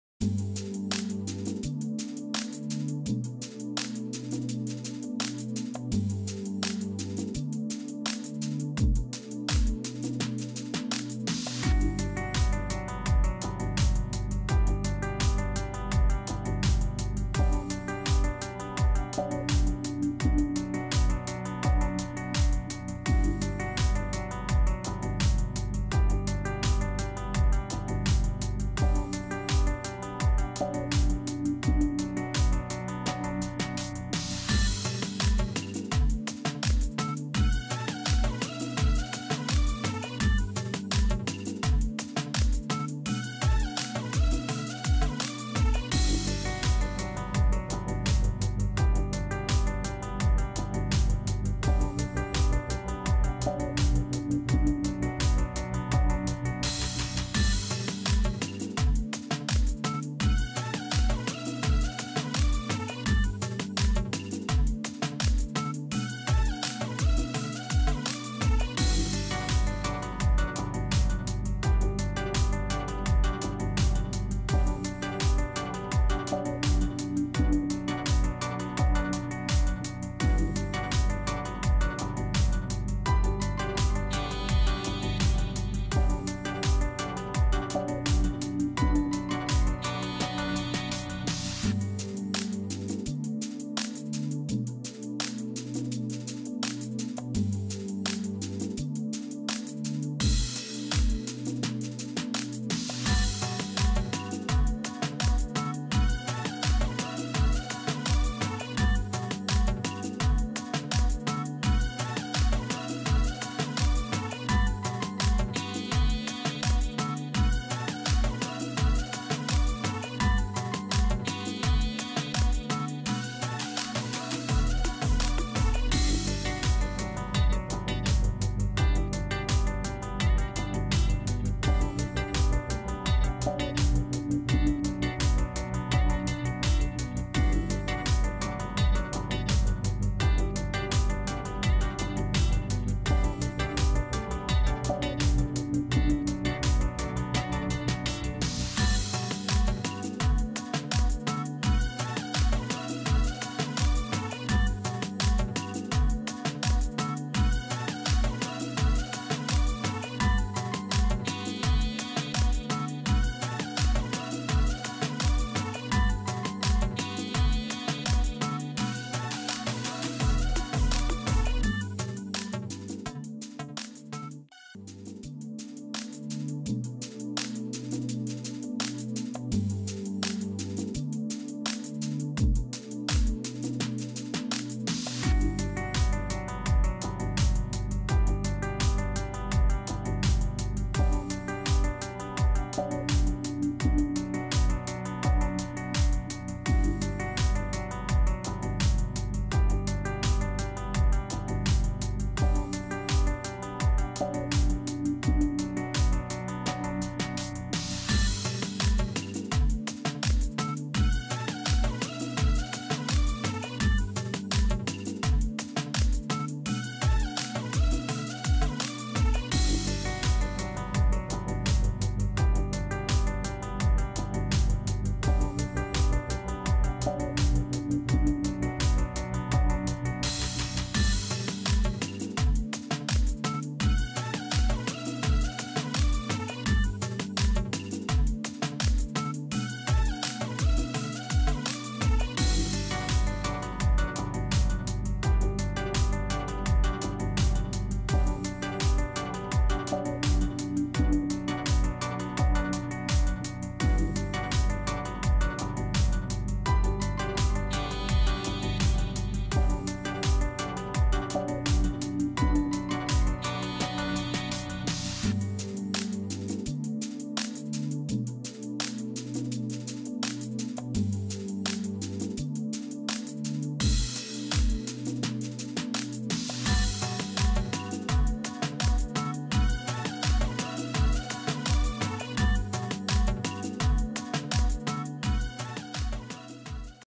Feathers. The process of watercolor drawing.
Hi guys! This video is something like a teaser for the next lesson. For the next lesson, I'll describe in detail how to draw feathers. This time I tested my camera for recording in 4k. If you have a 4k monitor or TV, then you can view all the details.